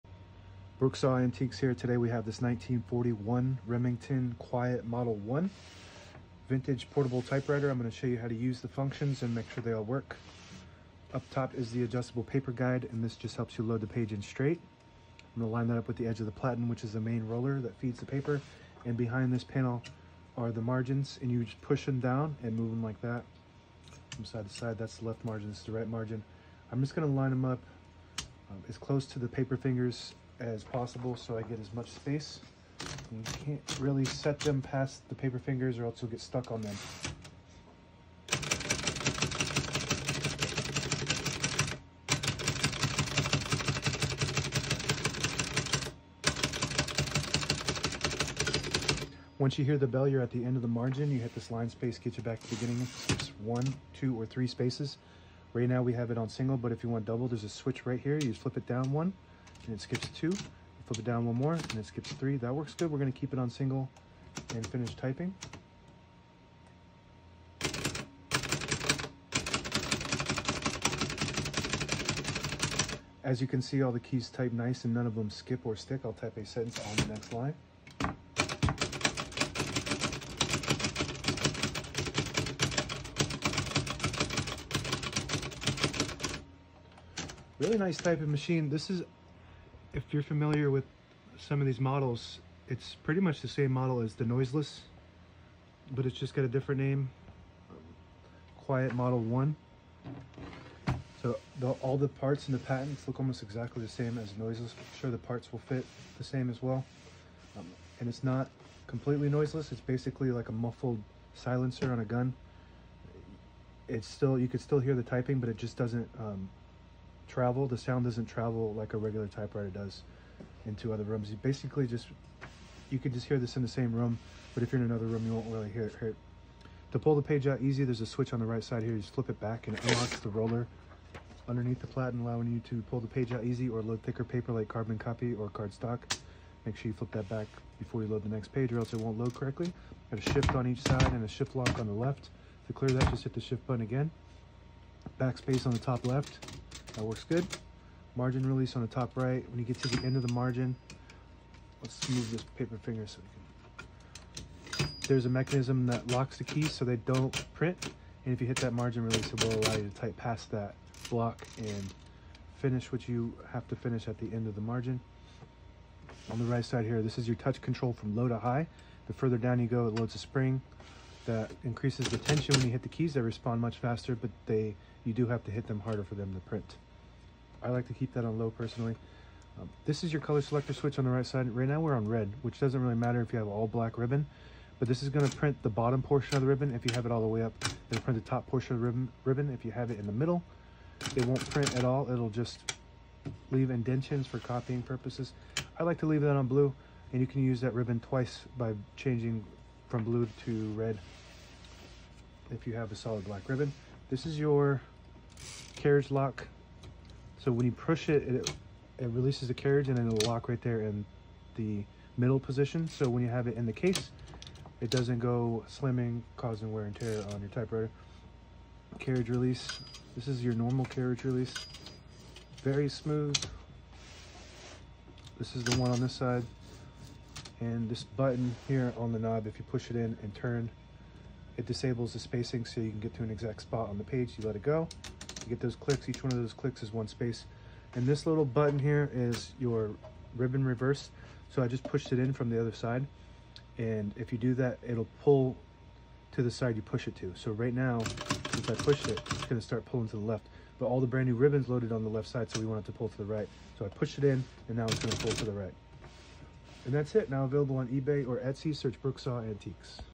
How to use all functions on a 1941 Remington Quiet Model 1 vintage portable typewriter
All functions work great 😍 now available on Etsy or eBay search Brooksaw Antiques

Links to my shop and similar typewriters available: